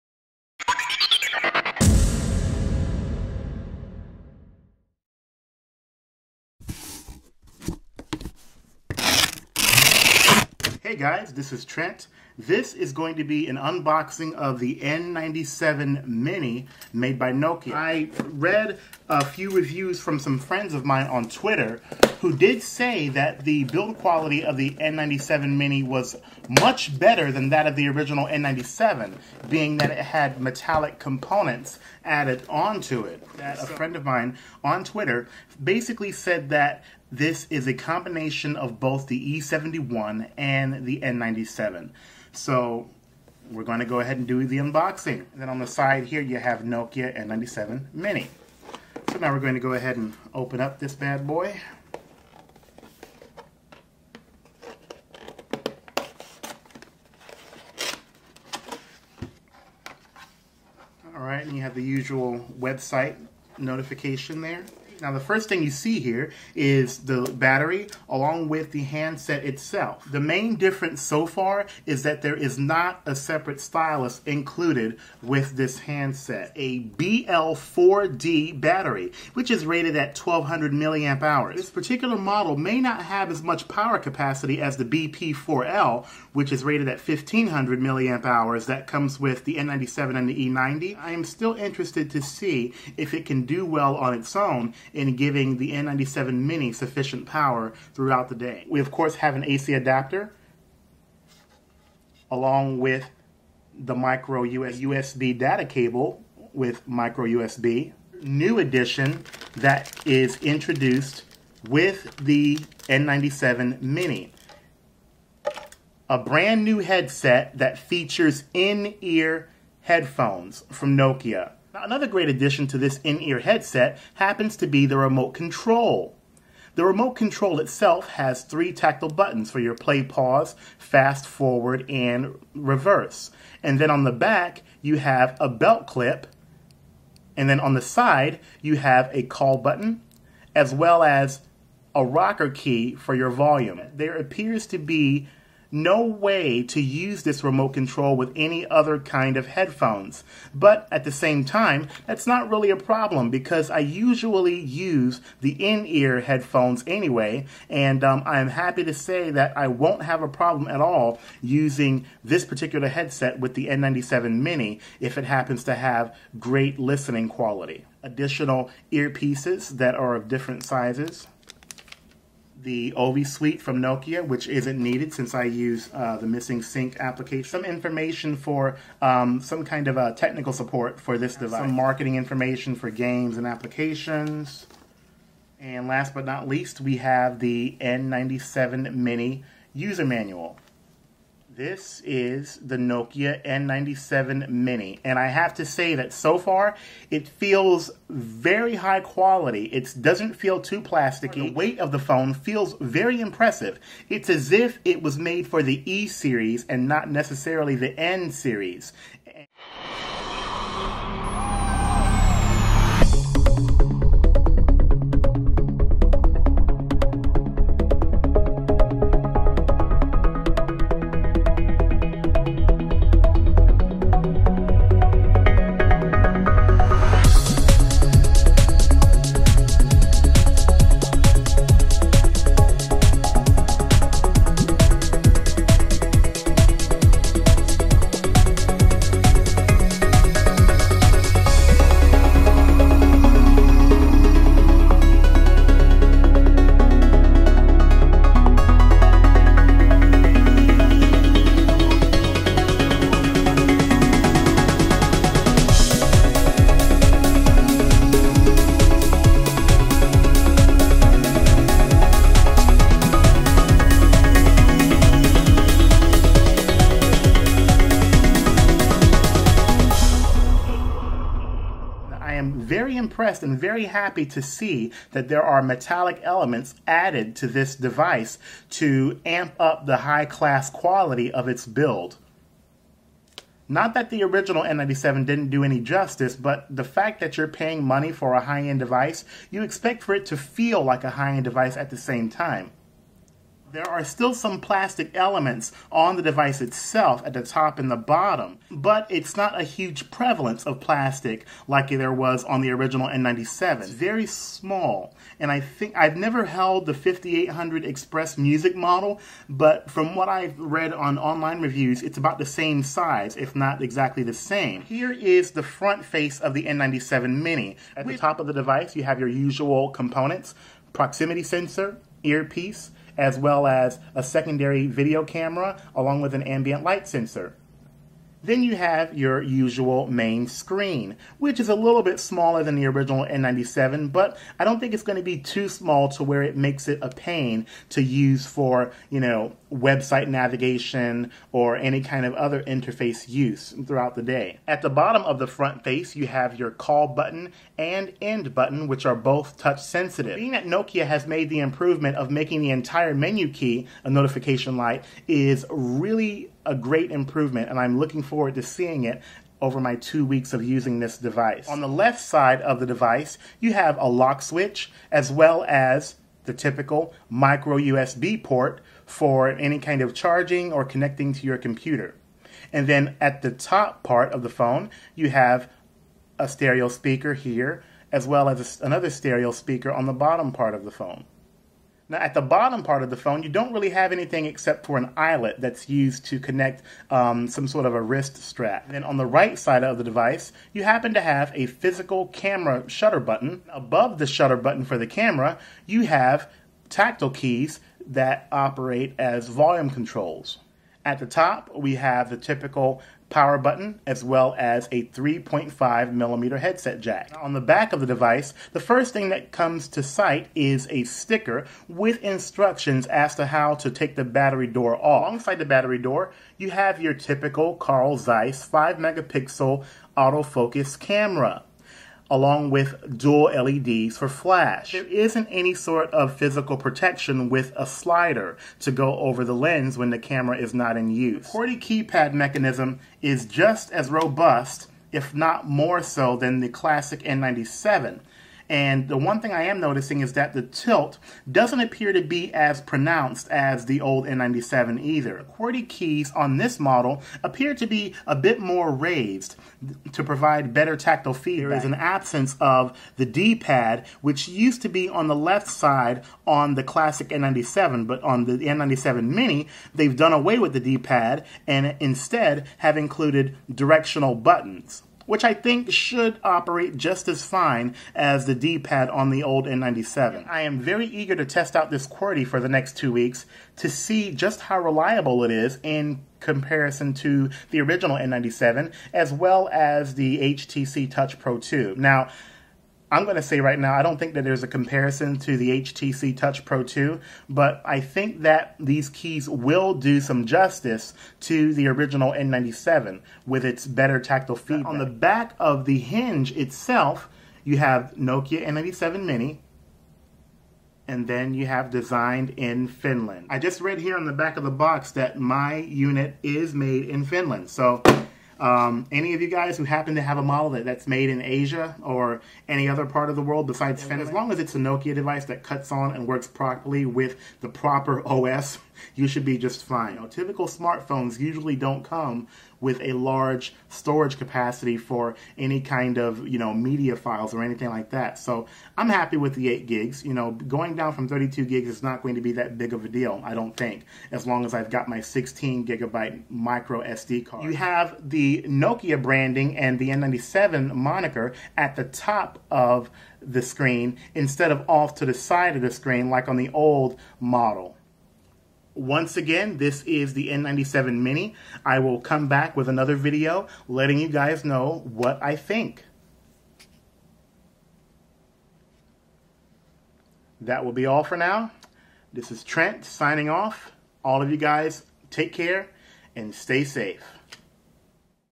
Nokia N97 Mini unboxing
This is an unboxing of the Nokia N97 Mini smartphone.